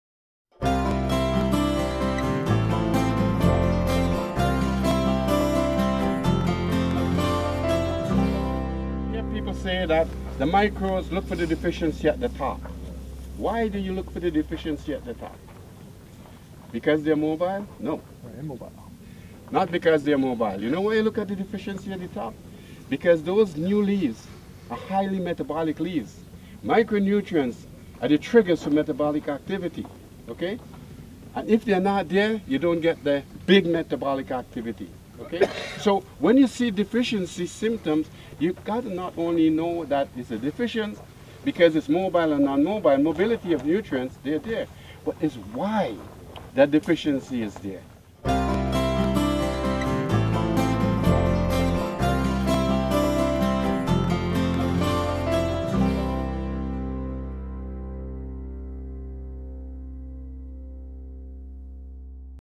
Diagnosing Micro Nutrient Deficiencies
Dr. Loraine Bailey talks with a crowed of producers at a field day in Southern Alberta.  the mobility Micro Nutrients is discussed